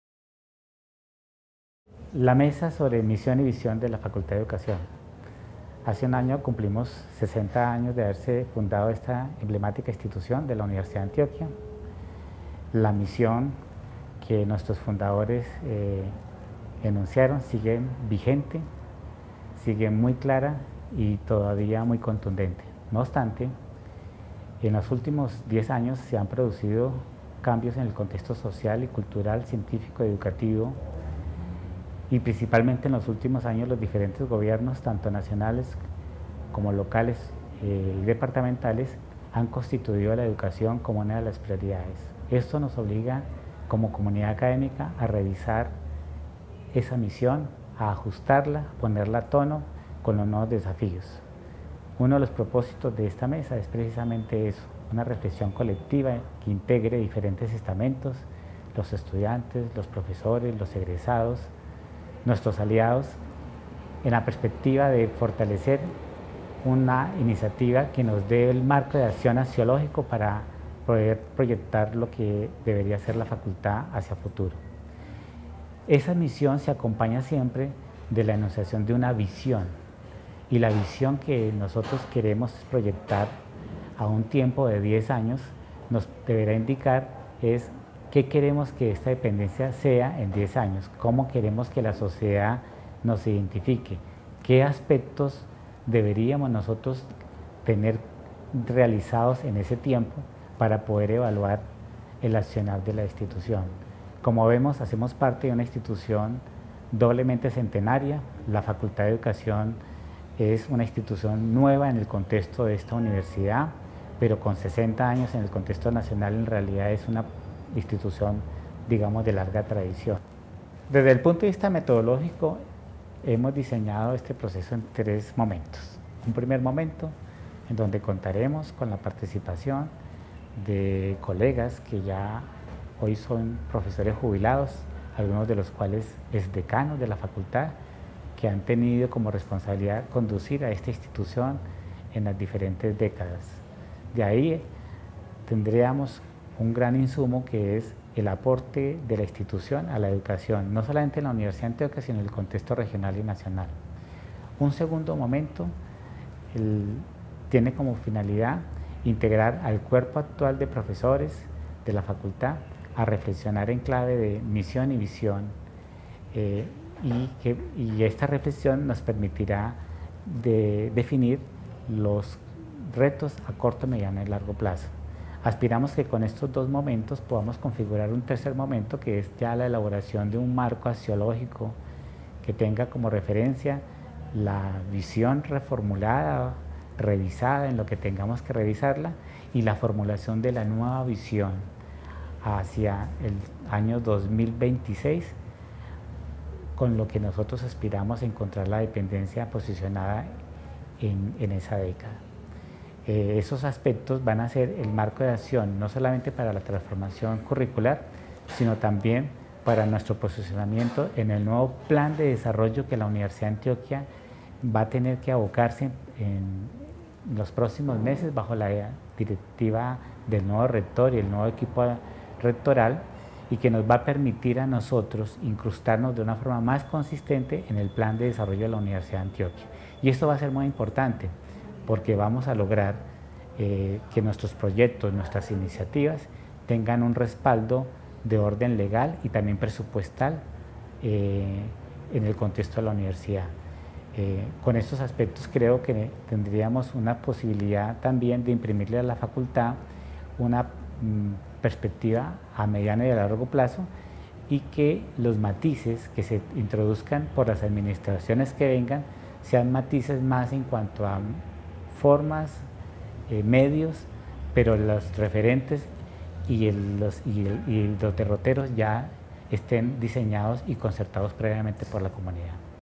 Transformación curricular - Mesa 1
Trabajo con los Docentes vinculados y ocasionales, con el fin de evaluar la Misión, visión y Propósitos  de formación de la Facultad a la luz de las tendencias nacionales en educación.